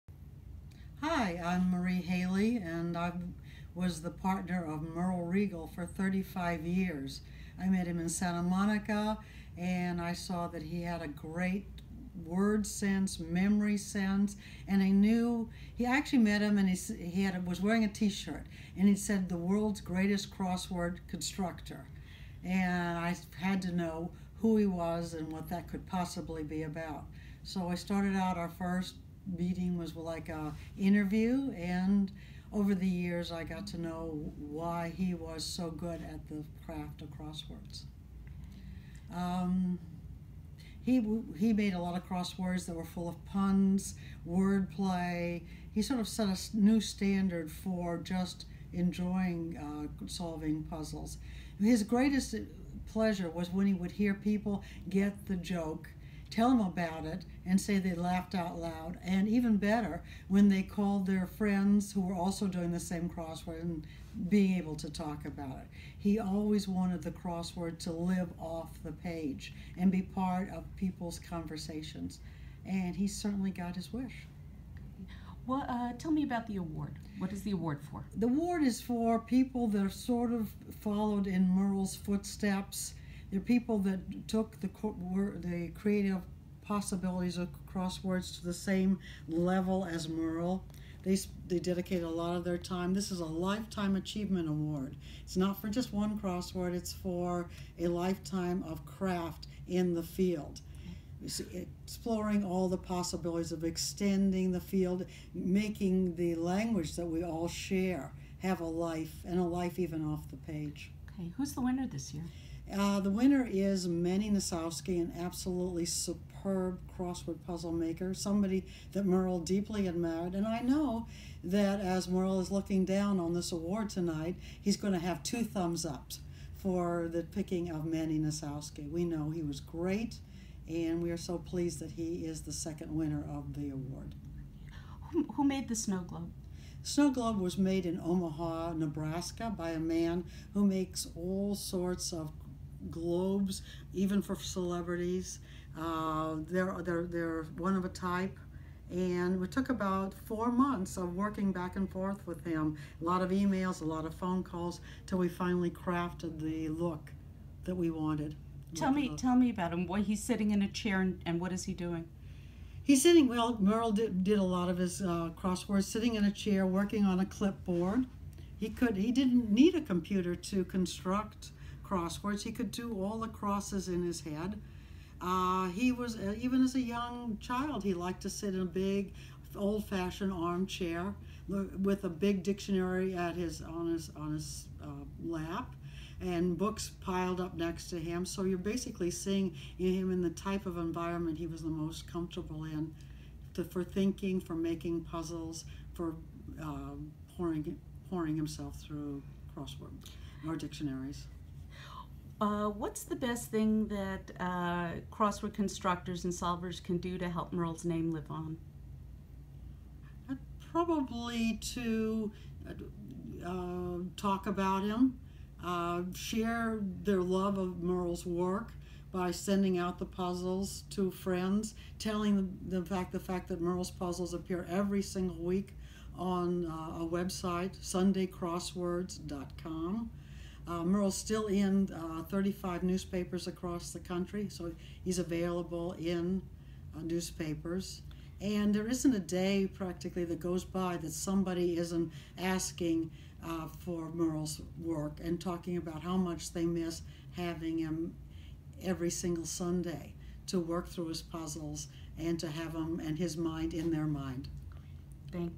Marie Haley on the 2017 Merl Reagle MEmoRiaL Award
Merl Reagle's partner, Marie Haley, talks about Merl, the MEmoRiaL Award and this year's recipient, constructor Manny Nosowsky.